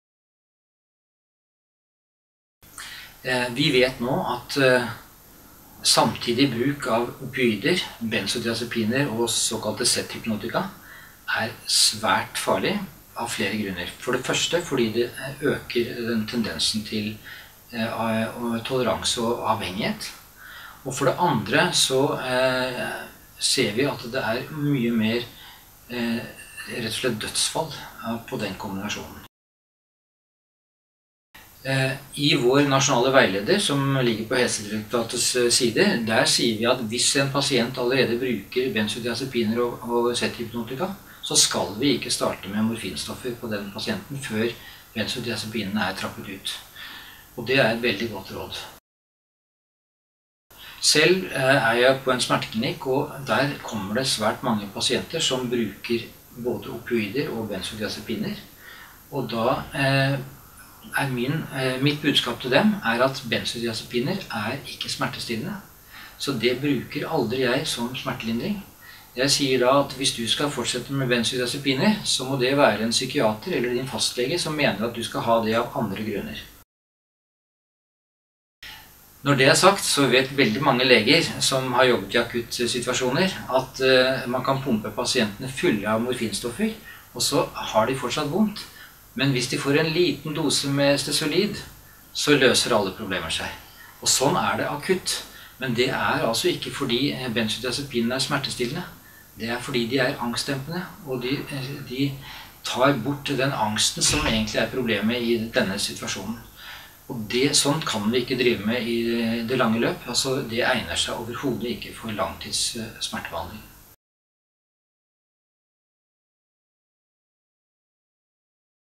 Audun Stubhaug: Samtidig bruk av ulike vanedannende medikamenter
Å bruke flere vanedannende legemidler samtidig er ekstra utfordrende, både med hensyn til skadelig bruk og forgiftningsfare. På den annen side kan det være at et lite tillegg med angstdempende medisin kan være det som skal til for å løse en situasjon hvor man vanskelig når til målet med bruk av bare smertestillende medisiner.